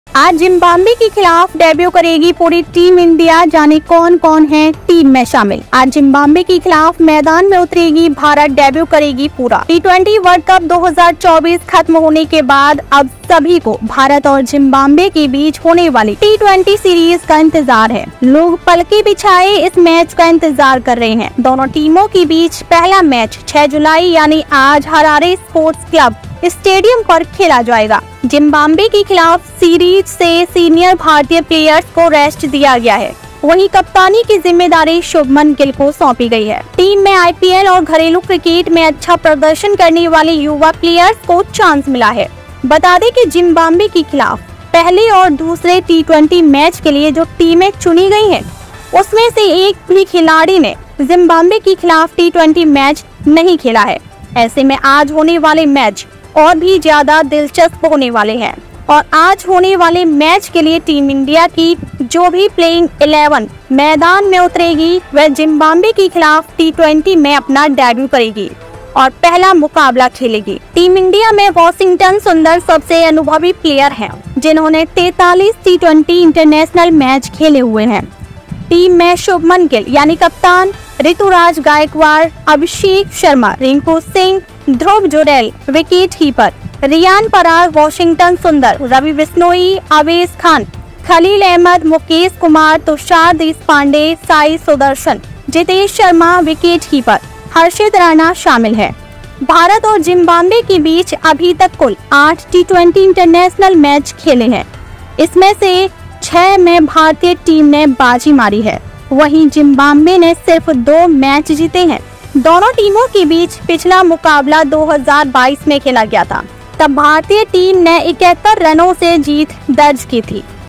IND vs ZIM: आज Zimbabwe के खिलाफ 'डेब्यू' करेगी पूरी Team India, जाने कौन कौन है टीम में शामिल
टी20 वर्ल्ड कप 2024 खत्म होने के बाद अब सभी को भारत और जिम्बाब्बे के बीच होने वाली टी20 सीरीज का इंतजार है। लोग पलके बिछाएं इस मैच का इंतजार कर रहे है। दोनों टीमों के बीच पहला मैच 6 जुलाई यानी आज हरारे स्पोर्ट्स क्लब स्टेडियम पर खेला जाएगा।जिम्बाब्वे के खिलाफ सीरीज से सीनियर भारतीय प्लेयर्स को रेस्ट दिया गया है। वहीं कप्तानी की जिम्मेदारी शुभमन गिल को सौंपी गई है।